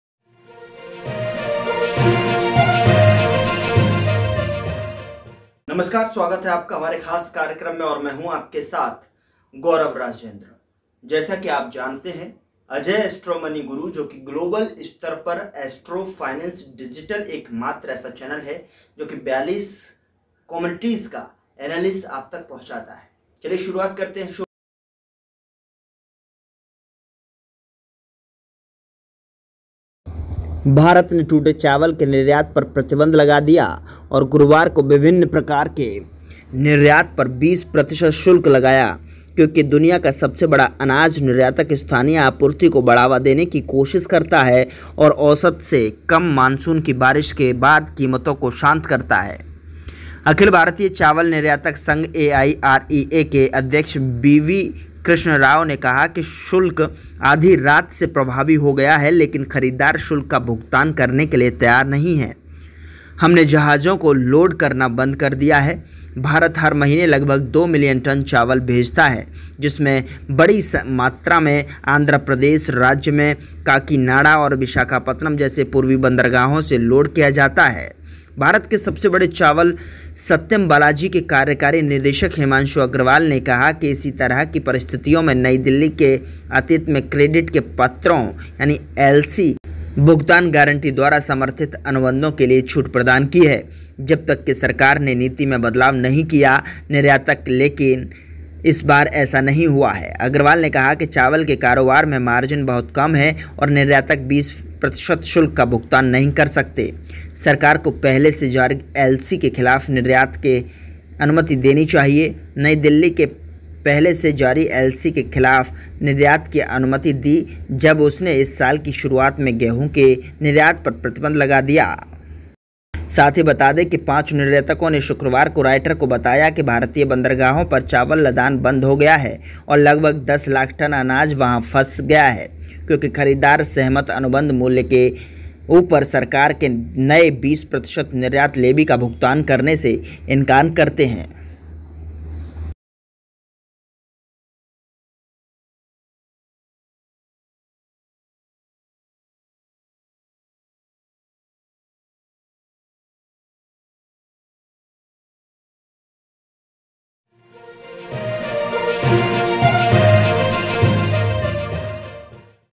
बंदरगाहों पर अटका 10 लाख टन अनाज, खरीदारों ने निर्यात शुल्क देने से किया इनकार
चीन का हो जाएगा बुरा हाल / भारत ने लगा दिया है चावल के निर्यात पर बैन/ Rice Export ban

बीज के लिए सरकार की तलाश के बीच निर्यातक भारतीय कपास के लिए तरस रहे हैं

श्रम की कमी ने मलेशिया को ताड़ के तेल के नुकसान के तीसरे वर्ष के लिए स्थापित किया

खाद्यान्नों को बढ़ाने के लिए केंद्र 3280 लाख टन का रखा लक्ष्य

भारत और चीन में, किसान सूखे और गर्मी से परेशान हैं क्योंकि चावल की फसल को खतरा है